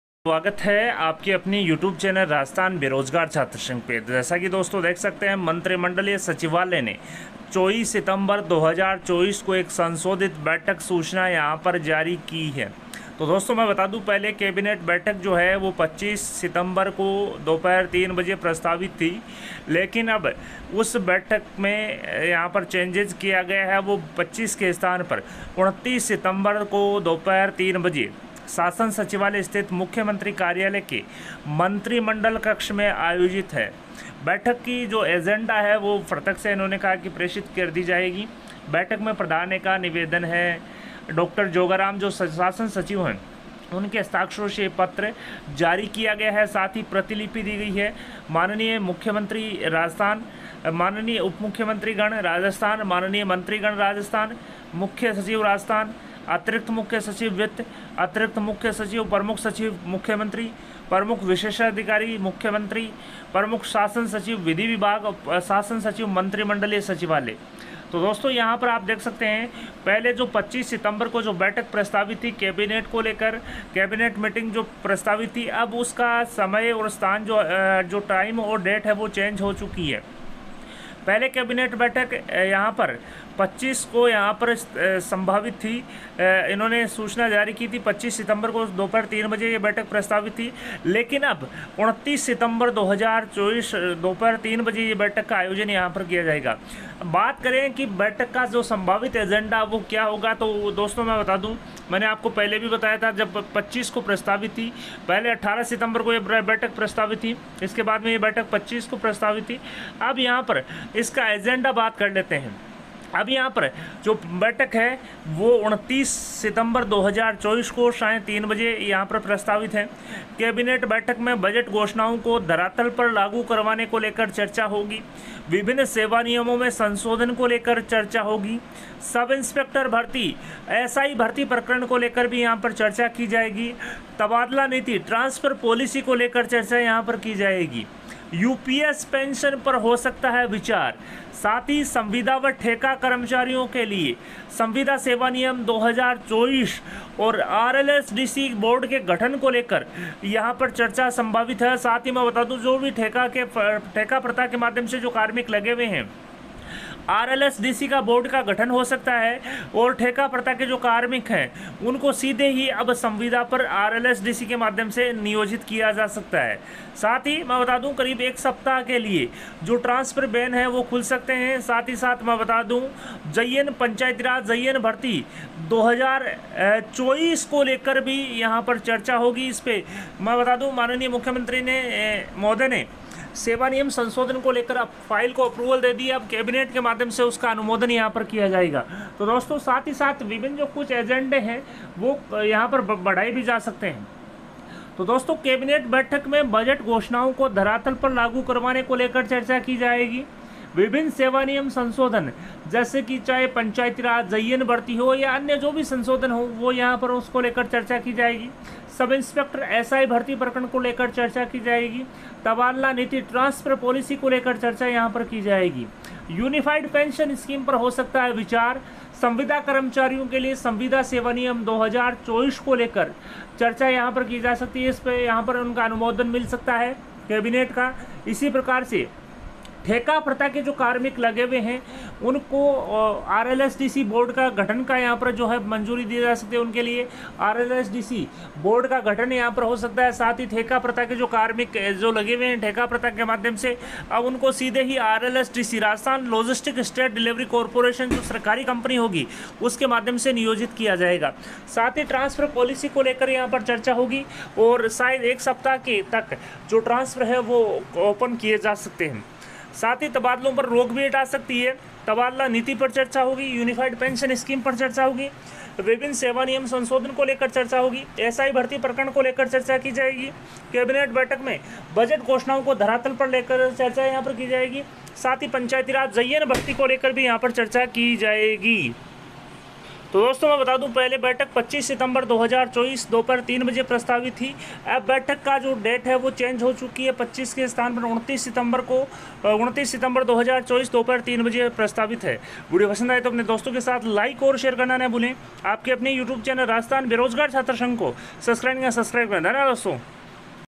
Cabinet Meeting @ 29 September, 2024 3 PM, ये हो सकते हैं सभावित Agenda ? देखे
Hello Friends,